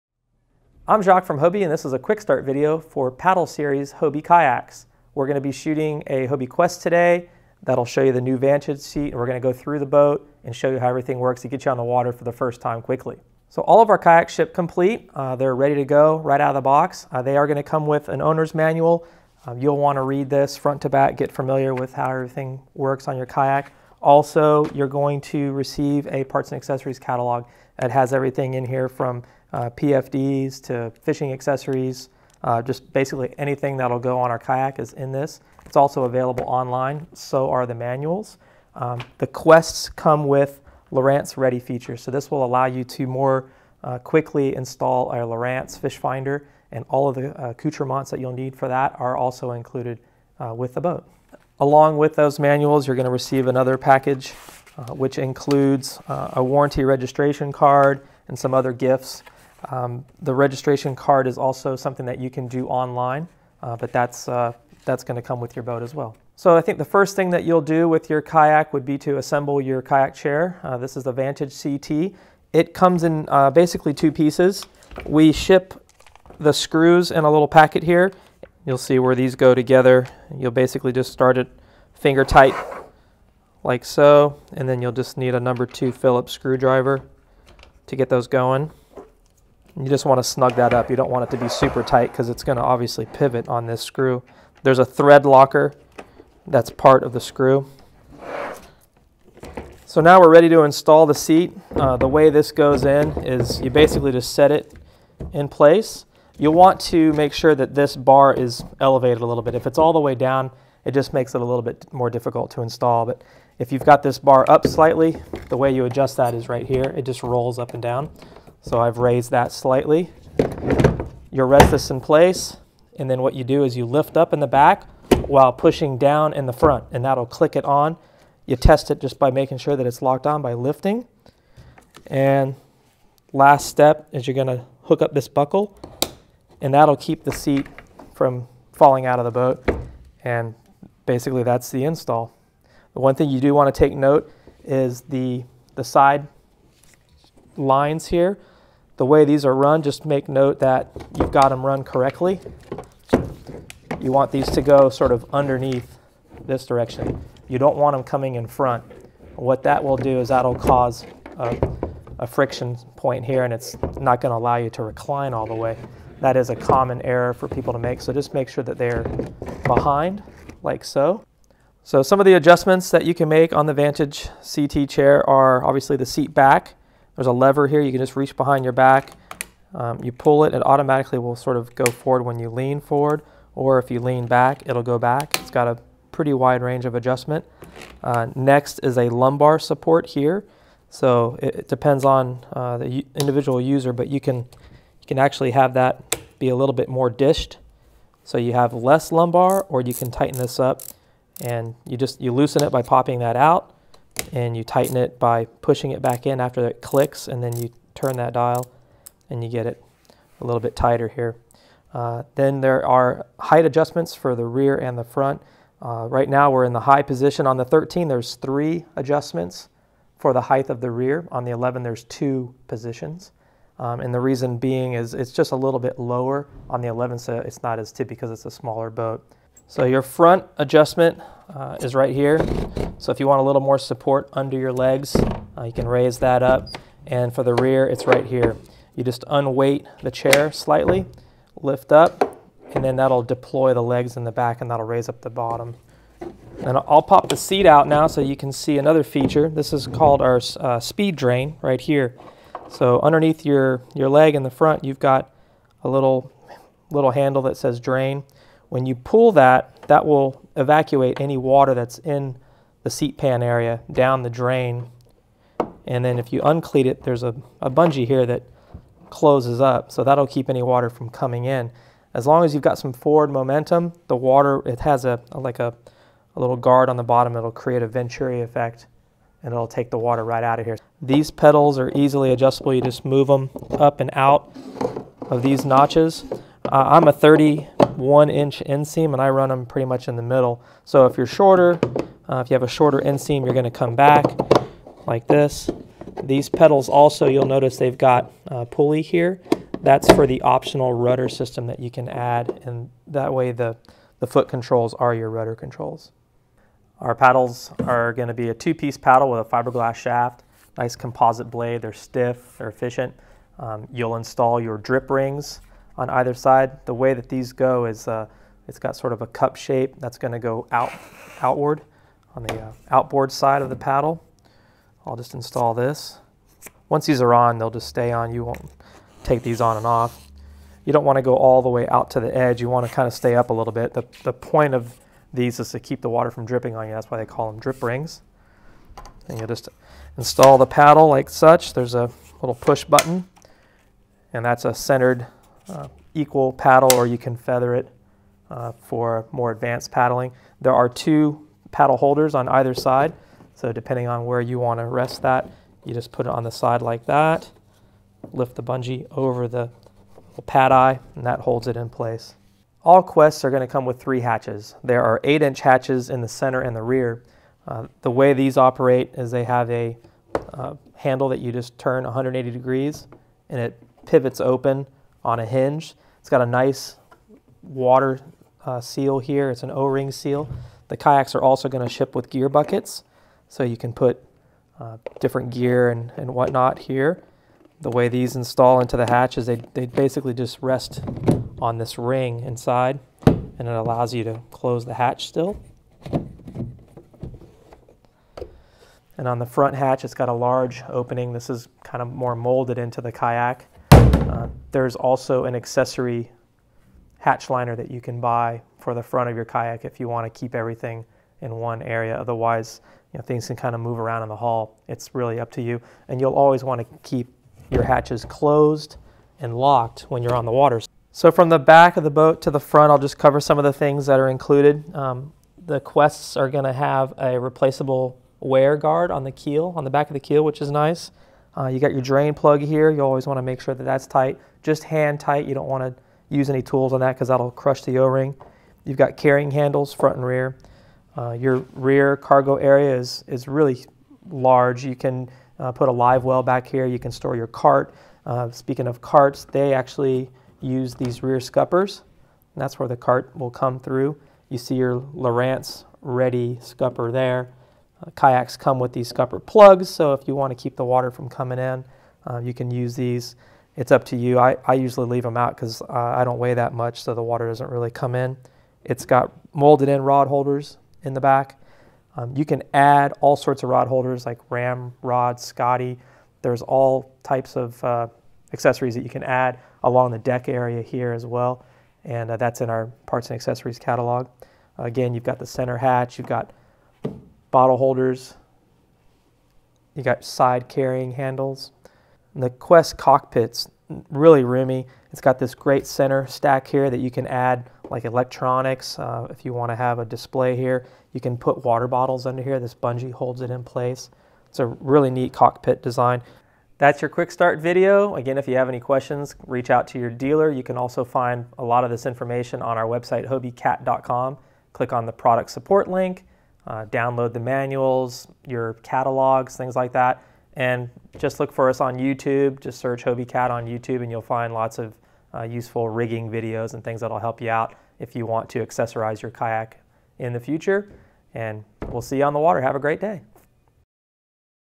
Hobie QuickStart for your Quest, 2017 & 2018 models with Vantage Seat.
Hobie QuickStart videos are intended to be a quick way to get you on the water with the basic info for setting up your kayak for the first time. This video covers installing the seatback, Seatback adjustments, Paddle setup, Footbrace setup, Hatches and cargo storage. This video is for all 2017+ Quest 11 and Quest 13 models.